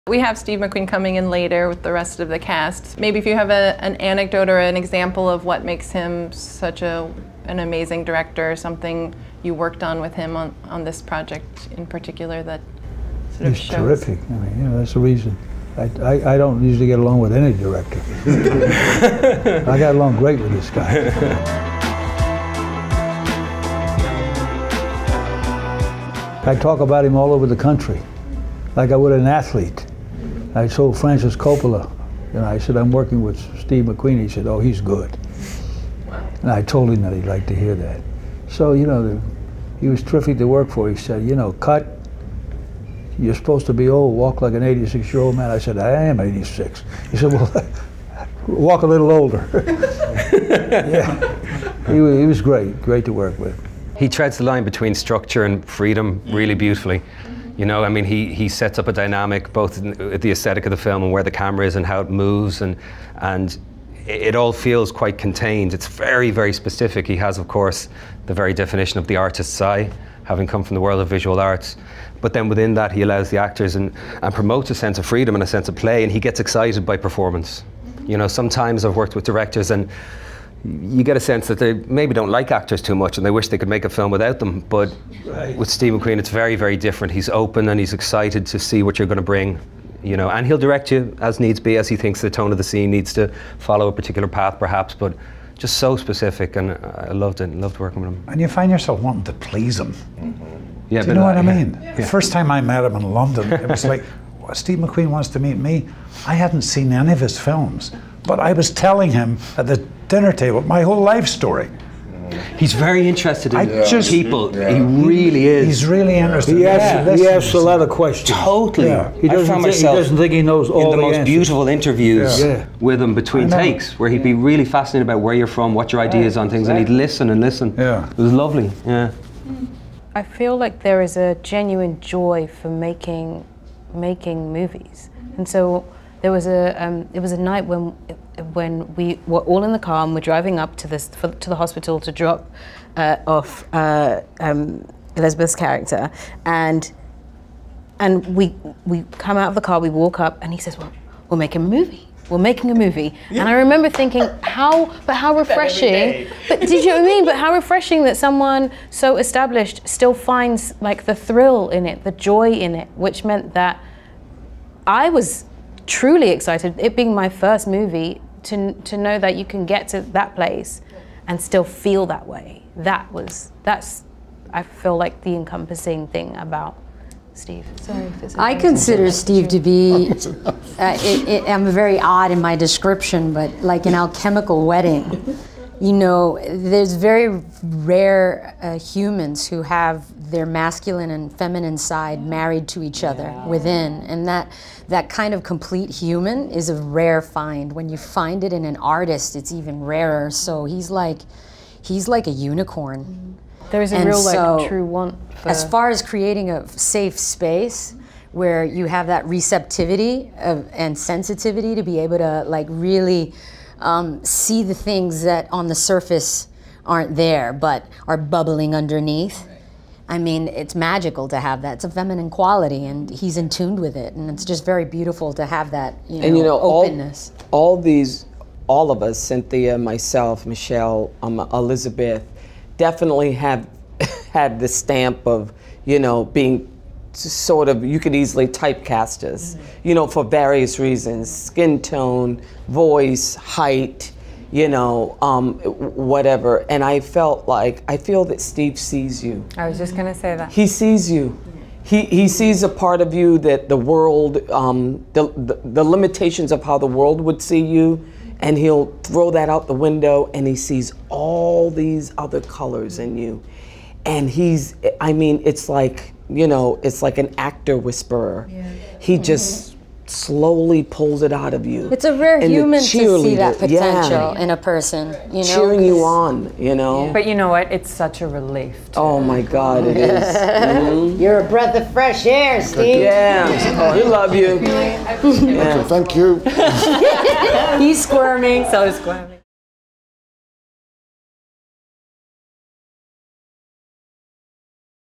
Liam Neeson, Cynthia Erivo, Colin Farrell, Viola Davis & More on Director Steve McQueen | TIFF 2018
Stars Robert Duvall, Colin Farrell, Liam Neeson, Cynthia Erivo, Michelle Rodriguez, and Viola Davis join THR at the 2018 Toronto International Film Festival to talk about working with director Steve McQueen on his new film 'Widows.'